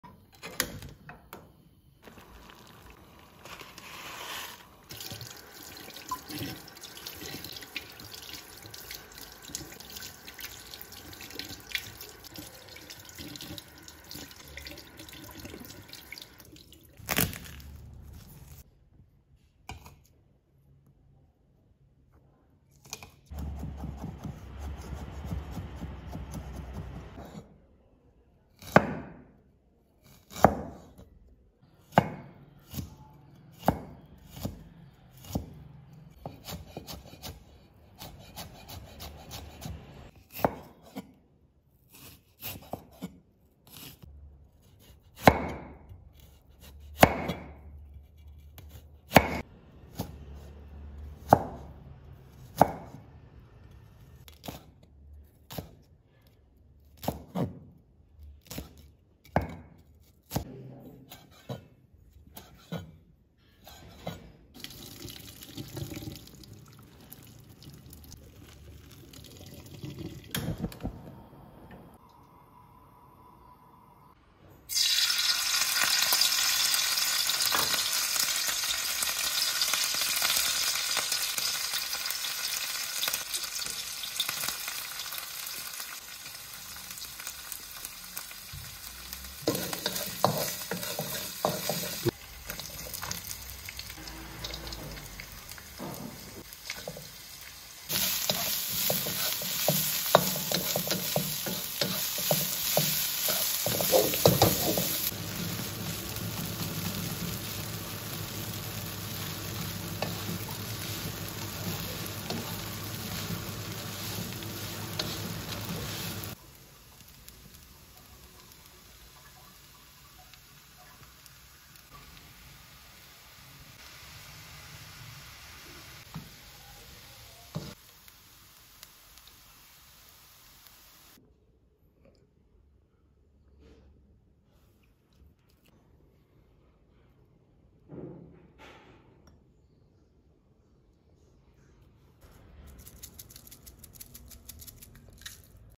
ASMR|Veg Mi Goreng|NO COMMENTARY
This is Veg Mie Goreng, Indonesian Fried Noodles completely veg for vegetarians. Comment for full recipe. Enjoy!